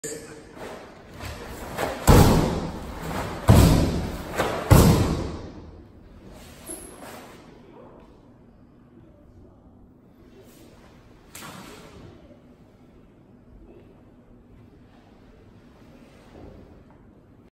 Arimoto-sensei demonstrating the ending of kata Bassai-sho
Sensei Koji Arimoto 4.dan JKS is here seen demonstrating the last part of kata bassai-sho at the JKS Honbu dojo in Nishi-Waseda, Tokyo, Japan on the 17th of October 2019